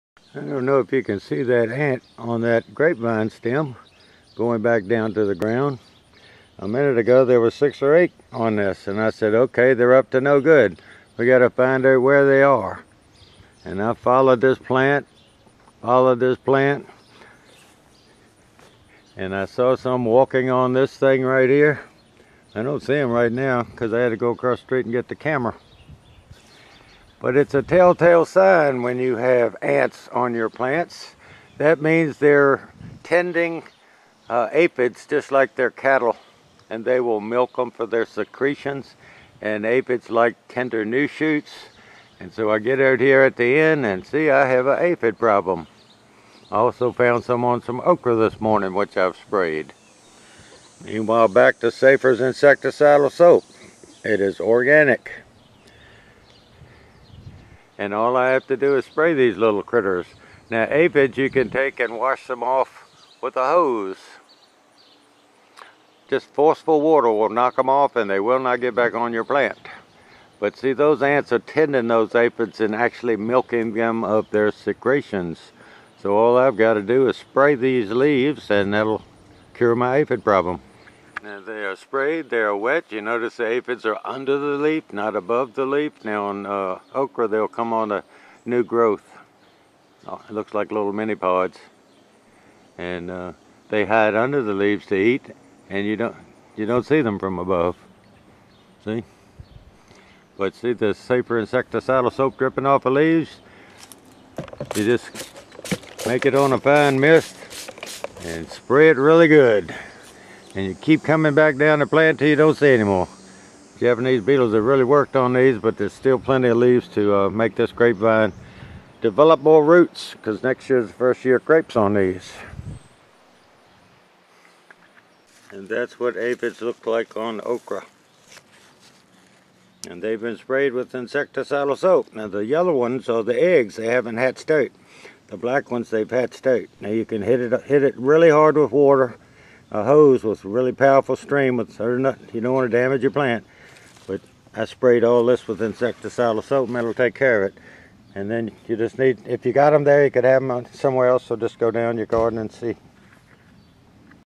ANTS AND APHIDS GO HAND IN HAND
While watering our grapevines at the river garden I noticed quite a few ants running up and down the vine stems. You can always find an ant somewhere but these seem to be in an hurry. Followed the ants and found out the reason why! APHIDS!